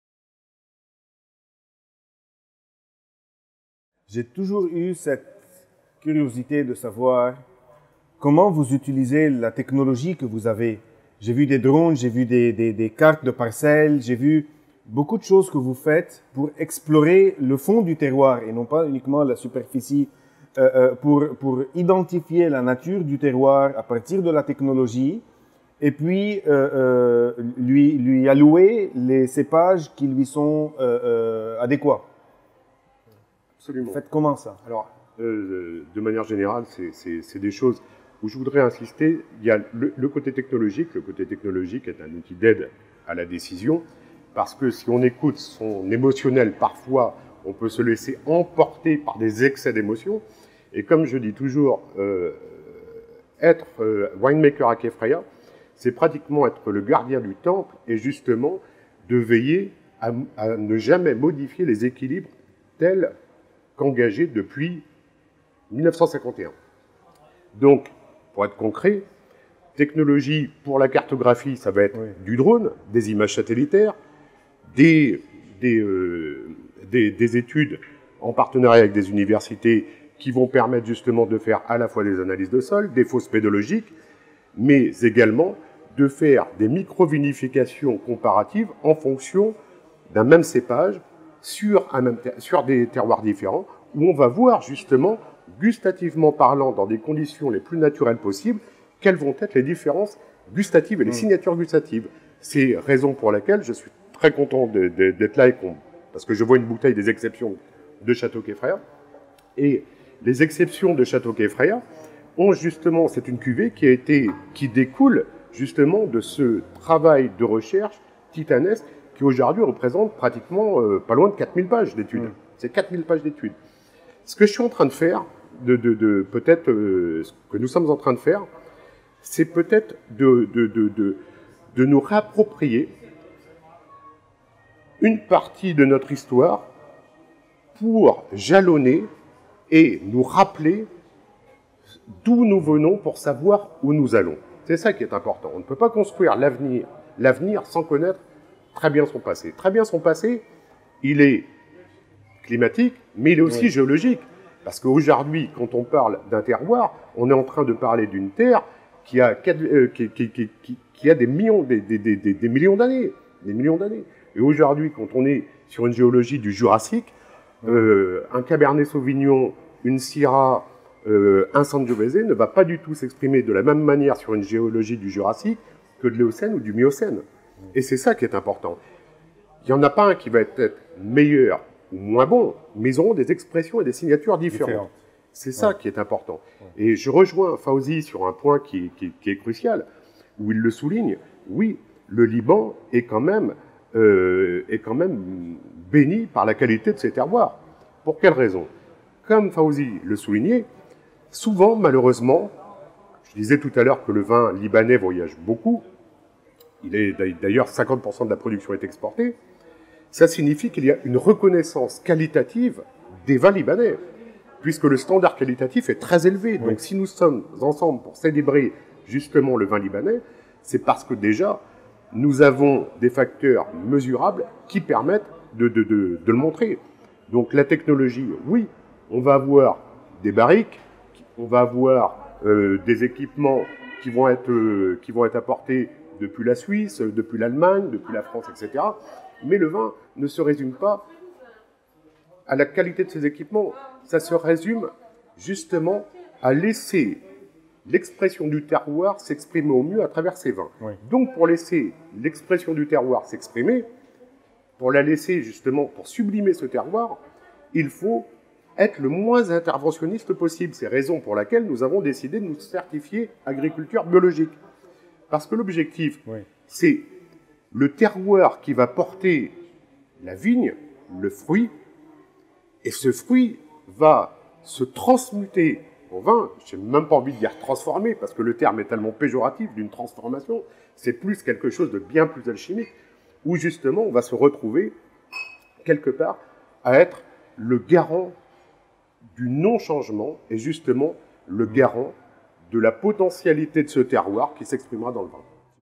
Chateau Kefraya: how to express the Lebanese identity through its terroir!
Fabrice Guiberteau Oenologist and Winemaker at Chateau Kefraya talks to us about their mission to express Lebanon's identity through their wines: how technology comes into place, how understanding the past and history of a terroir can help build its future, and how letting the terroir express itself is actually the truest way of expressing it's identity.